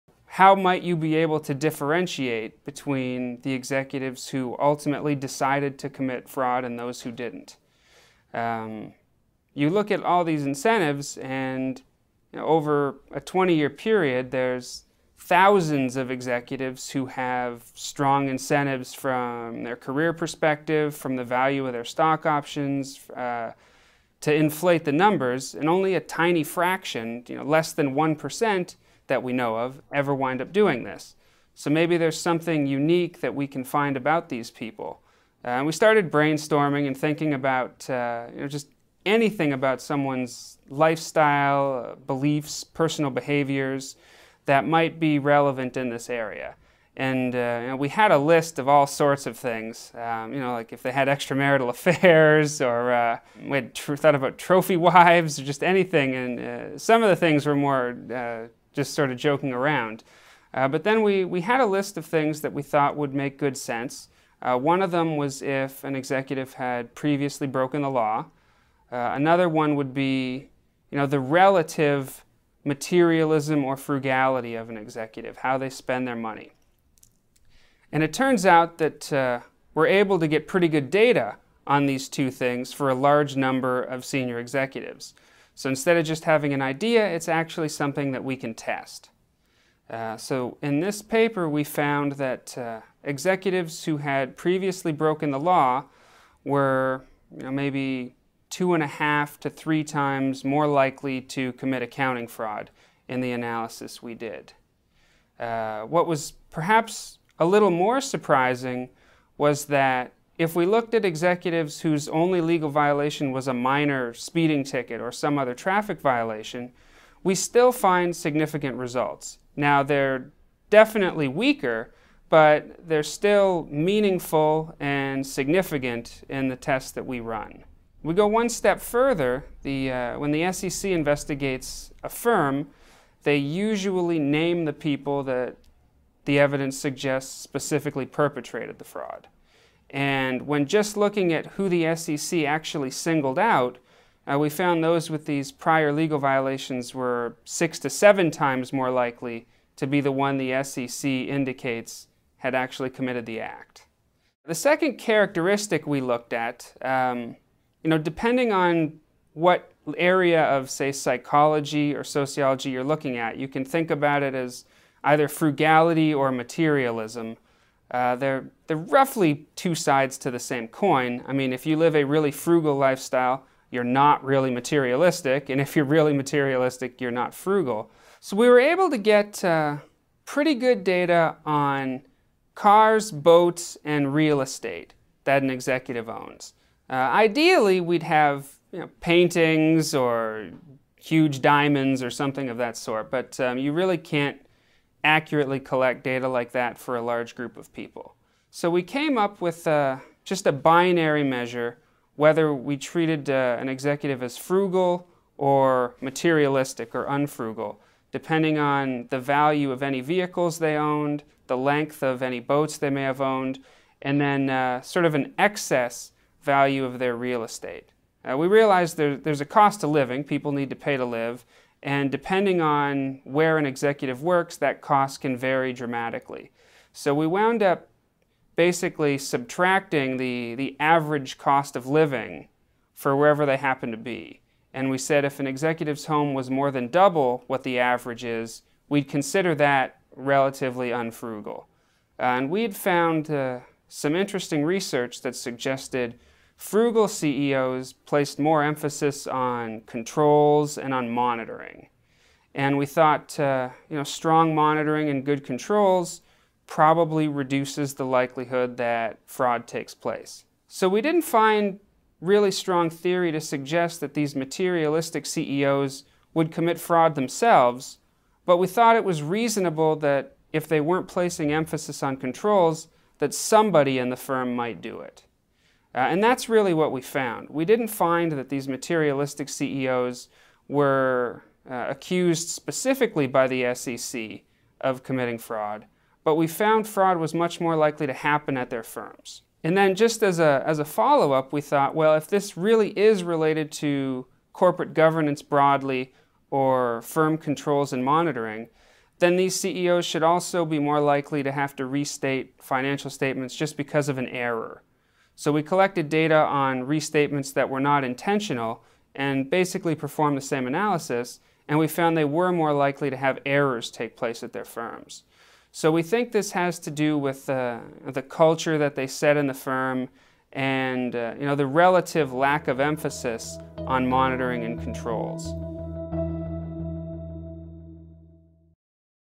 Predicting Corporate Fraud | Big Think
Predicting Corporate Fraud
Robert Davidson describes research he's conducted that may offer a way of predicting which corporations might commit fraud.
ASSISTANT PROFESSOR, GEORGETOWN UNIVERSITY:

Robert Davidson joined the faculty at Georgetown University in August 2011.  He completed his PhD and MBA at the University of Chicago in June 2011 and completed his undergraduate degree at Wayne State University in June 2003.
TRANSCRIPT:

Robert Davidson: How might you be able to differentiate between the executives who ultimately decided to commit fraud and those who didn't? You look at all these incentives and over a 20 year period there's thousands of executives who have strong incentives from their career perspective, from the value of their stock options to inflate the numbers, and only a tiny fraction, you know, less than one percent, that we know of, ever wind up doing this. So maybe there's something unique that we can find about these people.

We started brainstorming and thinking about just anything about someone's lifestyle, beliefs, personal behaviors that might be relevant in this area. And we had a list of all sorts of things like if they had extramarital affairs or we thought about trophy wives or just anything. And some of the things were more just sort of joking around. But then we had a list of things that we thought would make good sense. One of them was if an executive had previously broken the law. Another one would be the relative materialism or frugality of an executive; how they spend their money. And it turns out that we're able to get pretty good data on these two things for a large number of senior executives. So instead of just having an idea, it's actually something that we can test. So in this paper we found that executives who had previously broken the law were maybe two and a half to three times more likely to commit accounting fraud in the analysis we did.

What was perhaps a little more surprising was that if we looked at executives whose only legal violation was a minor speeding ticket or some other traffic violation, we still find significant results. Now, they're definitely weaker but they're still meaningful and significant in the tests that we run. We go one step further. When the SCC investigates a firm they usually name the people that the evidence suggests specifically perpetrated the fraud. And when just looking at who the SCC actually singled out, we found those with these prior legal violations were six to seven times more likely to be the one the SCC indicates had actually committed the act.

The second characteristic we looked at, depending on what area of say psychology or sociology you're looking at, you can think about it as either frugality or materialism, they're roughly two sides to the same coin. I mean if you live a really frugal lifestyle, you're not really materialistic, and if you're really materialistic you're not frugal.

So we were able to get pretty good data on cars, boats and real estate that an executive owns. Ideally we'd have paintings or huge diamonds or something of that sort but you really can't accurately collect data like that for a large group of people. So we came up with just a binary measure whether we treated an executive as frugal or materialistic or unfrugal depending on the value of any vehicles they owned, the length of any boats they may have owned and then sort of an excess value of their real estate. We realized there's a cost to living. People need to pay to live. And depending on where an executive works that cost can vary dramatically. So we wound up basically subtracting the average cost of living for wherever they happen to be and we said if an executive's home was more than double what the average is, we'd consider that relatively unfrugal.

We'd found some interesting research that suggested frugal CEOs placed more emphasis on controls and on monitoring. And we thought strong monitoring and good controls probably reduces the likelihood that fraud takes place. So we didn't find a really strong theory to suggest that these materialistic CEOs would commit fraud themselves, but we thought it was reasonable that if they weren't placing emphasis on controls that somebody in the firm might do it. And that's really what we found. We didn't find that these materialistic CEOs were accused specifically by the SCC of committing fraud, but we found fraud was much more likely to happen at their firms.